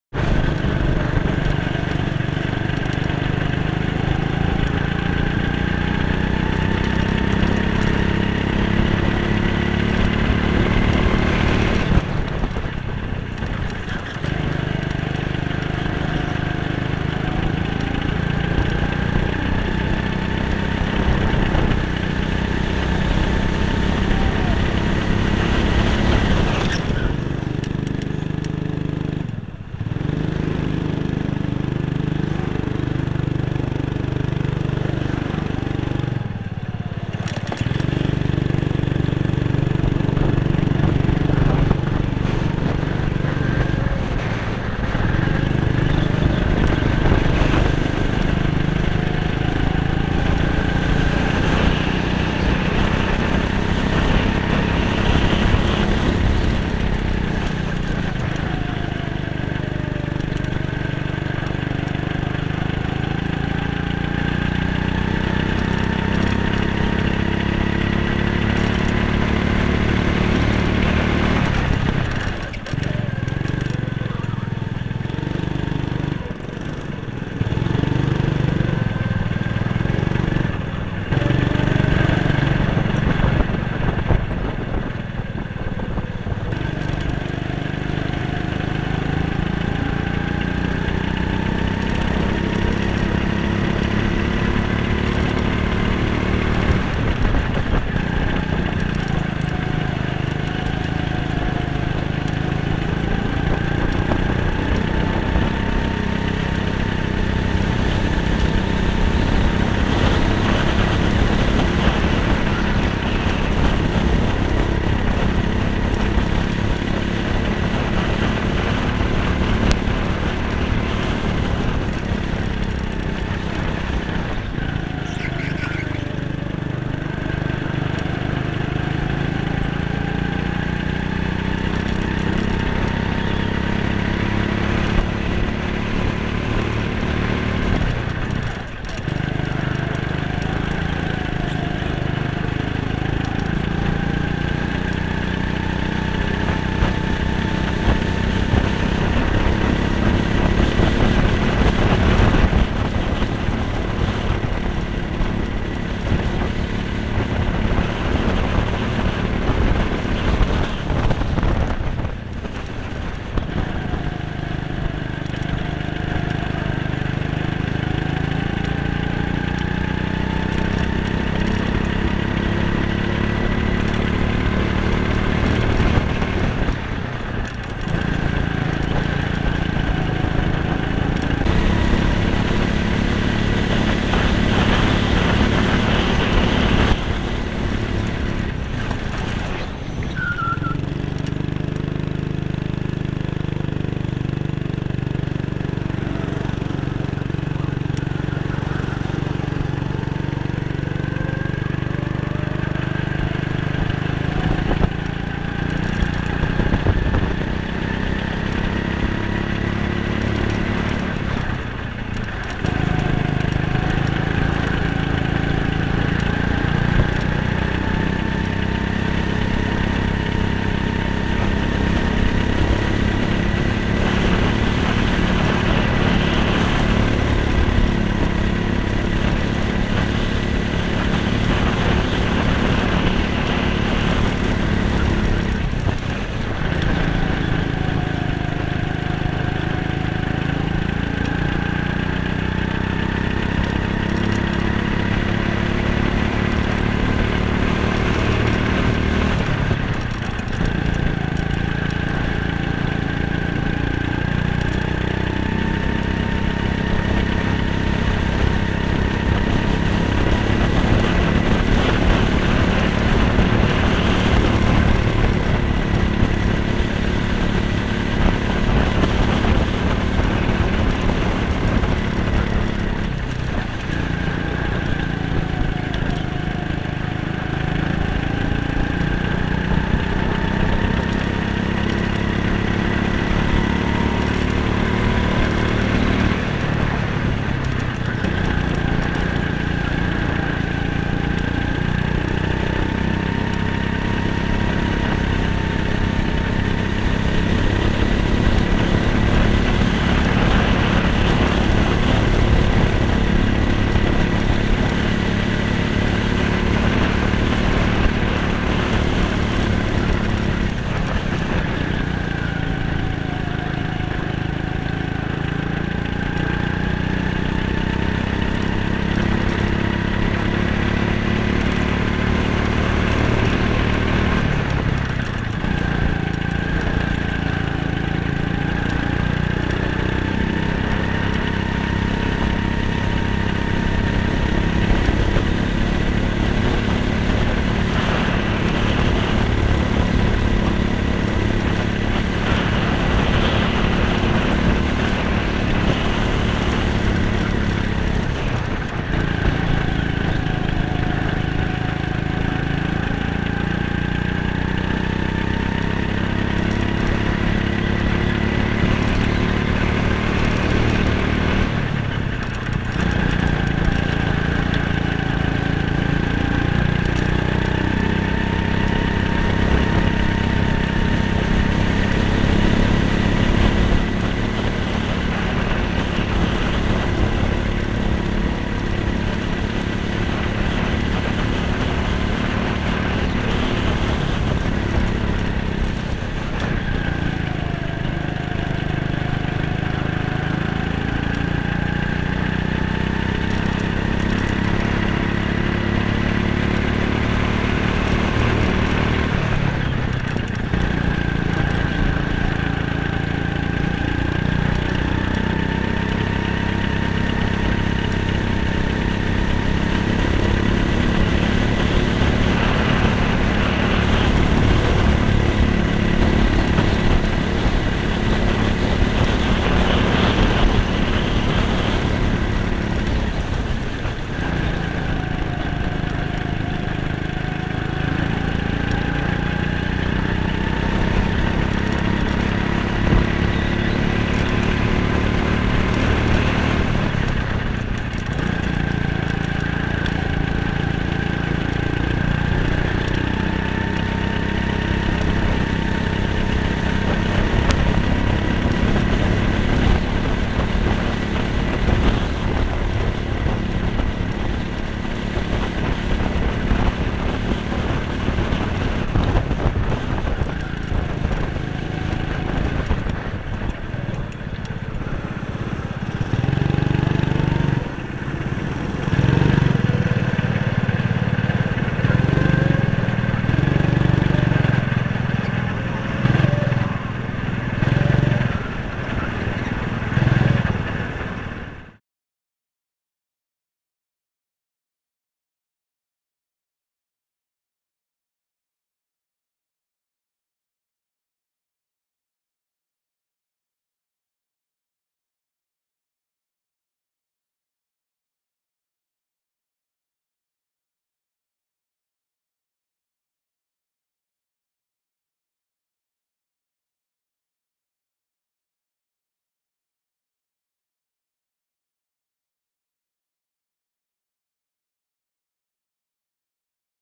Brens Bucks
Saturday night at the Go-Karts.  Great bucks party for Bren and he was on fire.  Fasted time for the month!!!